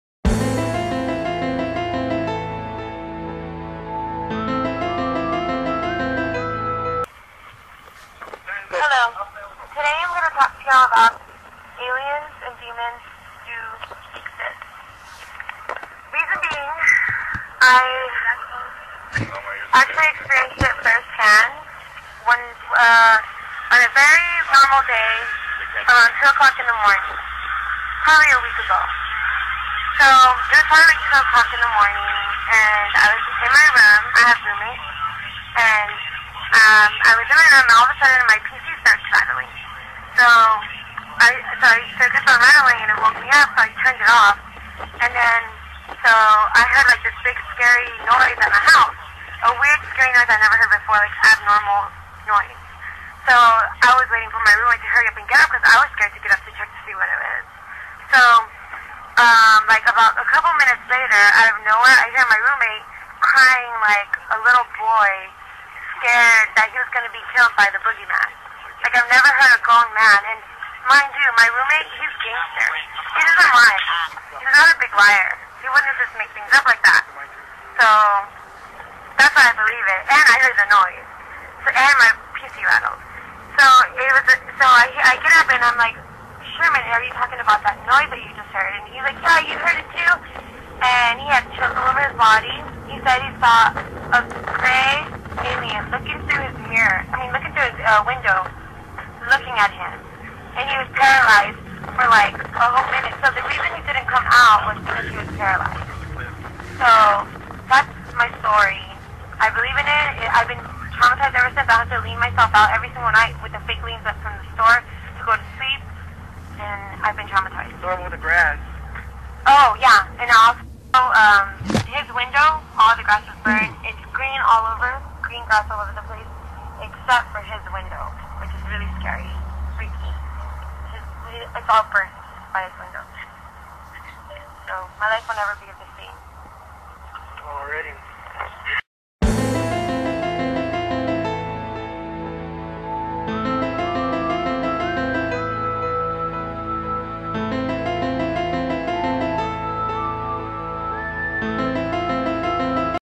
Sharon's Alien Encounter
My friend Sharon shares her alien encounter w/us during break @ work. I know the audio & video are kinda off my bad first video & my phone sucks @ recoding videos.